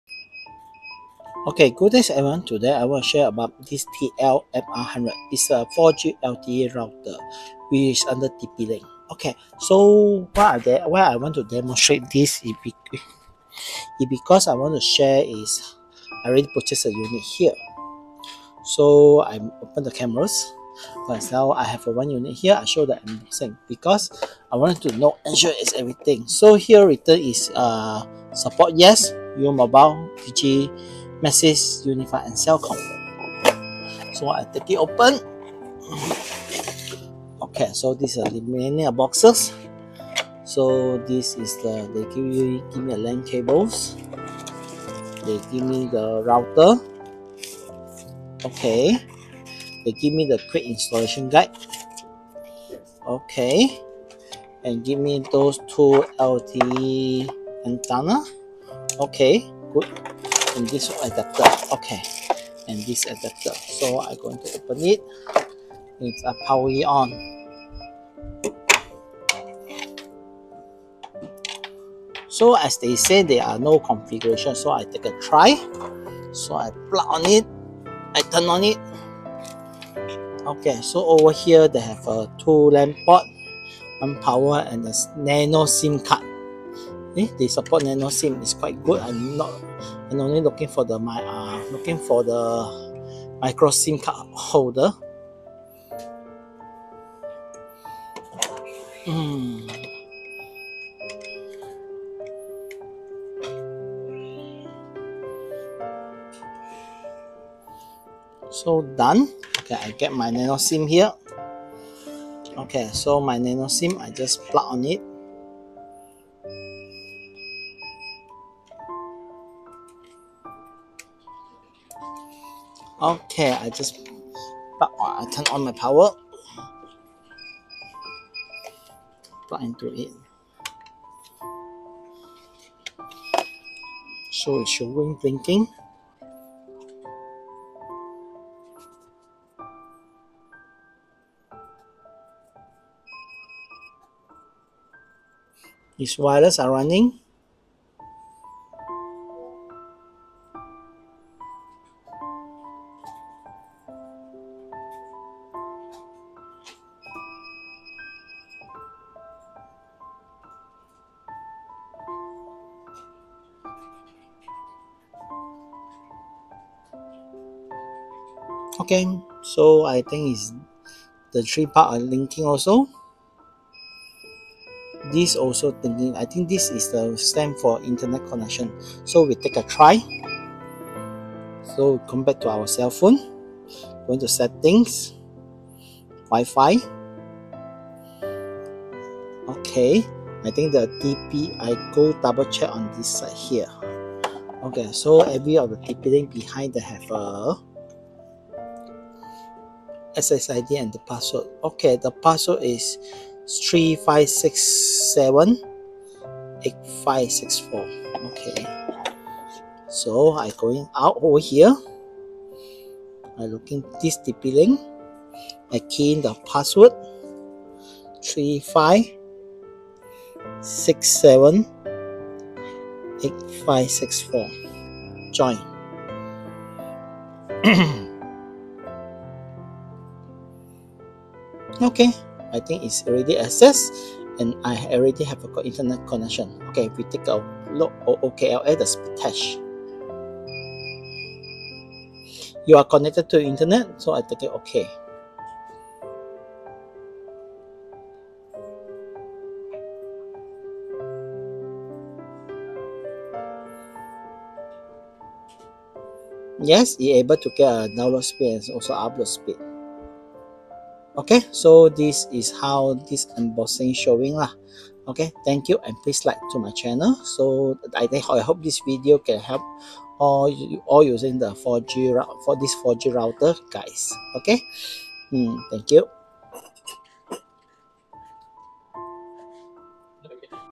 TP-Link: TL-MR100 4G Nano Sim Card Router Unboxing & Plug n Play Setup
This video just to show unboxing and plug n play setup